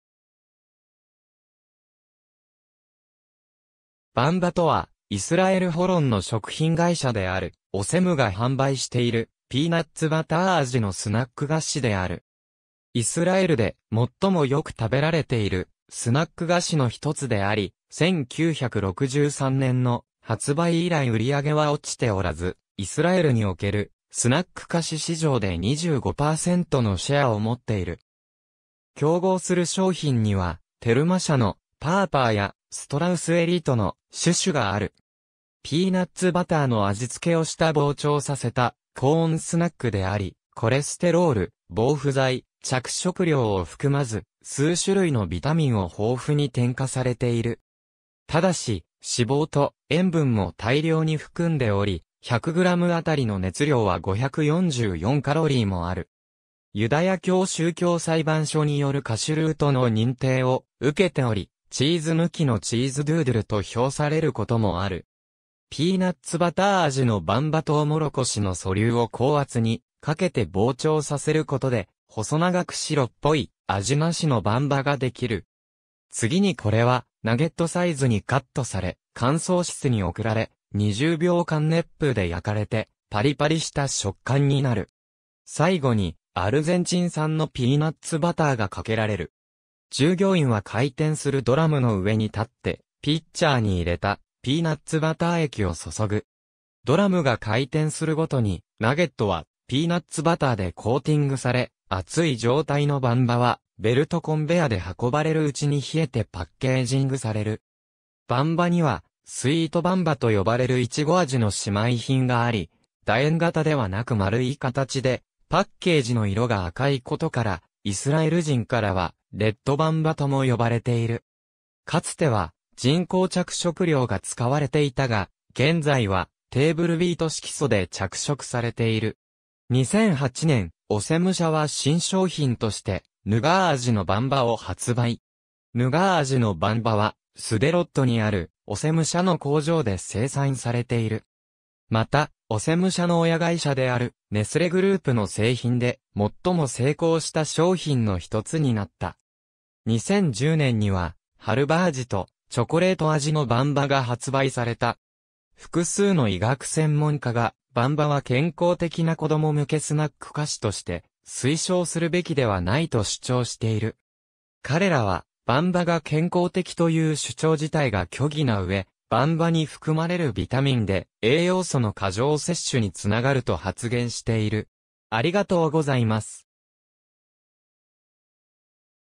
バンバ (スナック)
バンバ (ヘブライ語: בַּמְבָּה‎)とはイスラエル・ホロンの食品会社であるオセムが販売しているピーナッツバター味のスナック菓子である。
イスラエルで最もよく食べられているスナック菓子の一つであり、1963年の発売以来売り上げは落ちておらず、イスラエルにおけるスナック菓子市場で25%のシェアを持っている。
競合する商品にはテルマ社（2000年以来ユニリーバ子会社）の「パーパー(ParPar)」やストラウス・エリートの「シュシュ(Shush)」がある。
ピーナッツバターの味付けをした膨張させたコーンスナックであり、コレステロール、防腐剤、着色料を含まず、数種類のビタミンを豊富に添加されている。
ただし、脂肪と塩分も大量に含んでおり、100グラムあたりの熱量は544カロリーもある。
ユダヤ教宗教裁判所によるカシュルートの認定を受けており、「チーズ抜きのチーズ・ドゥードゥル」と評されることもある。
ピーナッツバター味のバンバ トウモロコシの粗粒を高圧にかけて膨張させることで、細長く白っぽい、味無しのバンバができる。
次にこれはナゲットサイズにカットされ、乾燥室に送られ、20秒間熱風で焼かれてパリパリした食感になる。
最後にアルゼンチン産のピーナッツバターがかけられる。
従業員は回転するドラムの上に立って、ピッチャーに入れたピーナッツバター液を注ぐ。
ドラムが回転するごとにナゲットはピーナッツバターでコーティングされ、熱い状態のバンバはベルトコンベアで運ばれる内に冷えてパッケージングされる。
バンバにはスイートバンバ(ヘブライ語: במבה מתוקה‎)と呼ばれるイチゴ味の姉妹品があり、楕円形ではなく丸い形でパッケージの色が赤いことからイスラエル人からはレッドバンバ(ヘブライ語: במבה אדומה‎)とも呼ばれている。
かつては人工着色料が使われていたが、現在はテーブルビート色素で着色されている。
2008年、オセム社は新商品としてヌガー味のバンバを発売。
ヌガー味のバンバはスデロットにあるオセム社の工場で生産されている。
また、オセム社の親会社であるネスレグループの製品で最も成功した商品の一つになった。
2010年にはハルヴァ味とチョコレート味のバンバが発売された。
複数の医学専門家が、バンバは健康的な子供向けスナック菓子として推奨するべきではないと主張している。
彼らはバンバが健康的という主張自体が虚偽なうえ、バンバに含まれるビタミンで栄養素の過剰摂取につながると発言している。